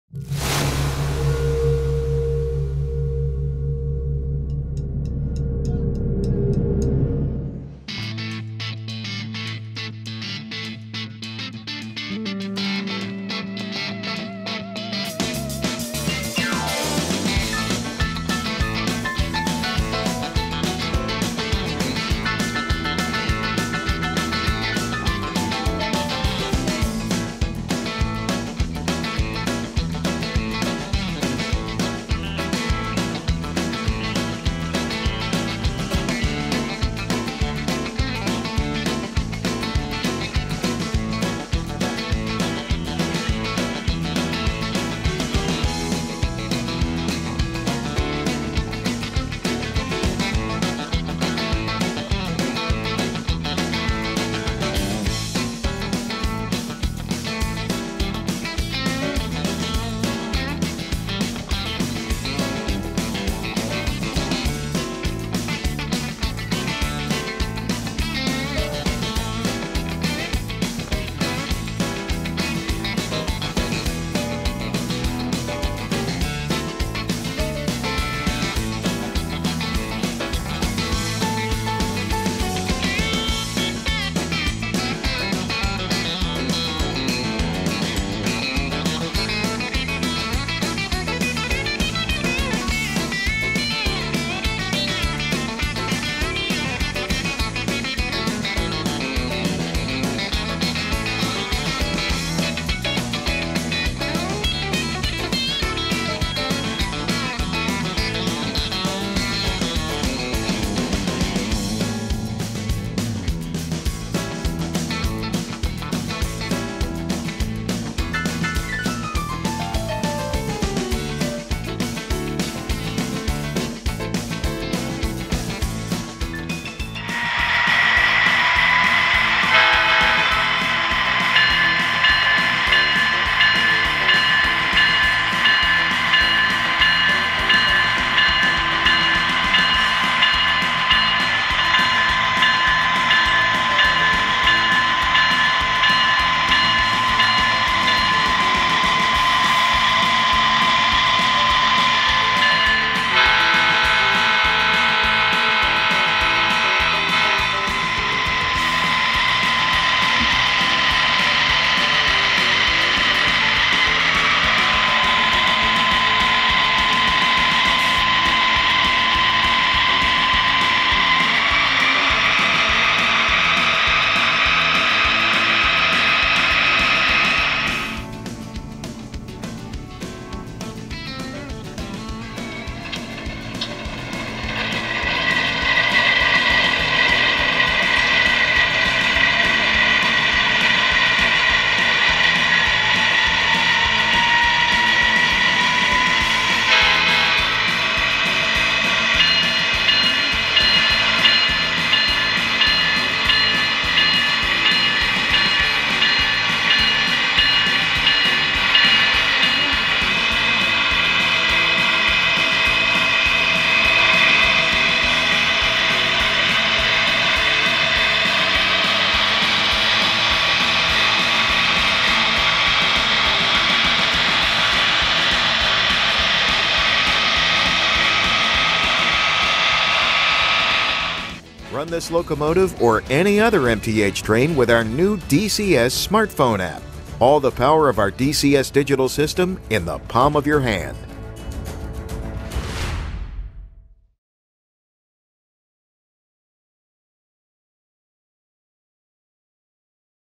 MTH 2020 HO ES44 Diesel Spotlight
Like all M.T.H. locomotives, our new ES44AC offers HO scale’s finest combination of detail, realism, and performance. Listen to the chant of an authentic GEVO-12 motor in the Proto-Sound 3 versions, and throttle down as low as three scale miles per hour so you can admire the details as the Evolution Series glides by: see-through fans and grilles, walkways with safety tread, flashing ditch lights, windshield wipers, body and truck air brake piping, and a host of other separately applied details. Activate the couplers from anywhere on the layout using any DCC controller or the DCS Digital Command System.  For the ultimate in power and sound, operate these diesels in multiple-unit lashups under DCC, or DCS digital command control. If you’re running under the M.T.H. DCS system, you can double- or even triple-head the ES44AC with virtually any combination of 3.0 locomotives.

GE claims the EVOs are “the most fuel-efficient, most  environmentally friendly diesel locomotives in history… If every freight train in North America were pulled by an Evolution Series Locomotive, the reduction of smog-producing pollutants would be like removing 48 million cars from the road each year.” Everything about these locomotives has been examined, questioned, and re-thought, generating 25 new U.S. patents in the process.  Before they went on sale in 2005, 52 preproduction units were tested in revenue service across the continent for more than a year. Every Big Six railroad has ponied up to buy the ES44, with the BNSF currently rostering the largest EVO fleet.  Around the world, over 3,700 Evolution Series diesels are operating in 10 countries.